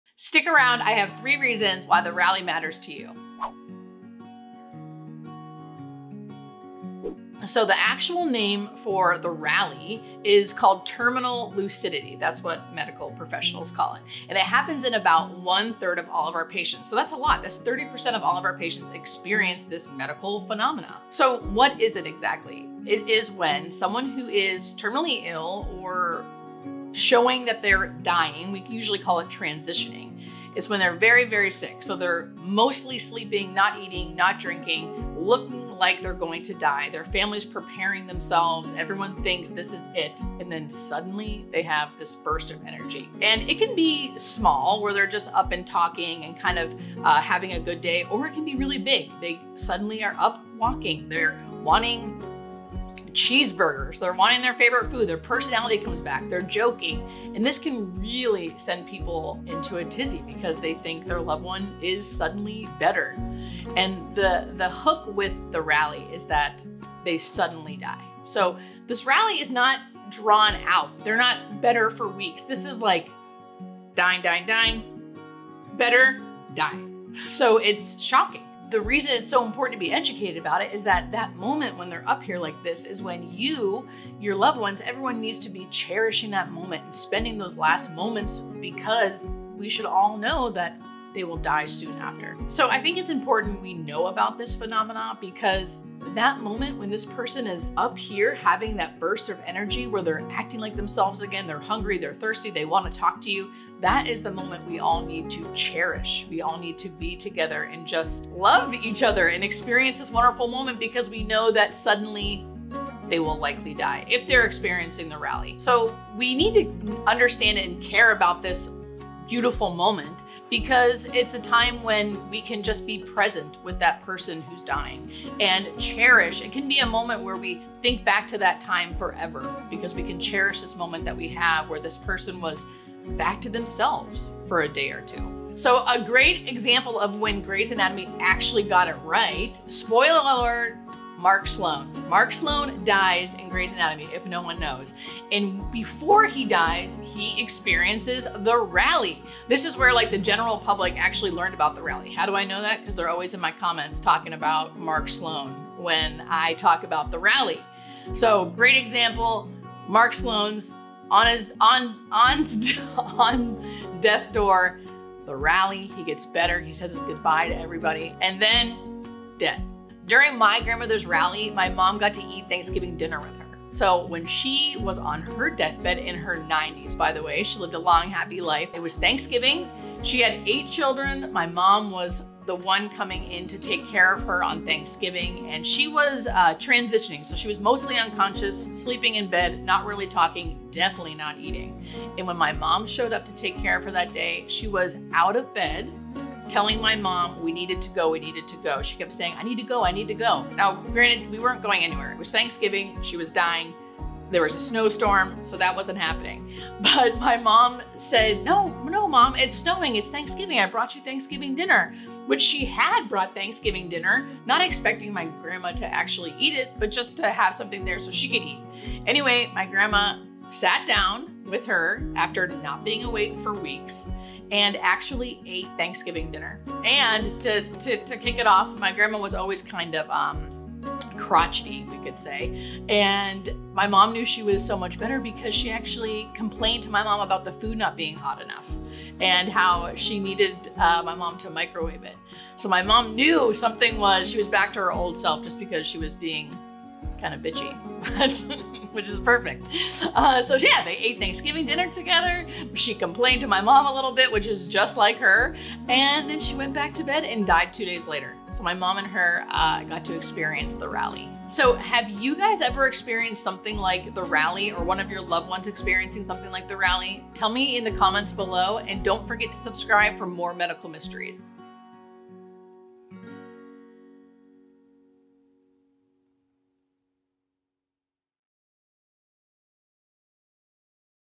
Explaining Terminal Lucidity: The Rally
I'm back with something I get asked about all the time! THE RALLY! What is it? What is Terminal Lucidity? Why do people experience it? What is that famous rally that everyone always mentions! Let's talk about what Terminal Lucidity is and why it's such an interesting phenomena.

When dealing with any medically related events or medical emergencies, please communicate with your primary health care provider.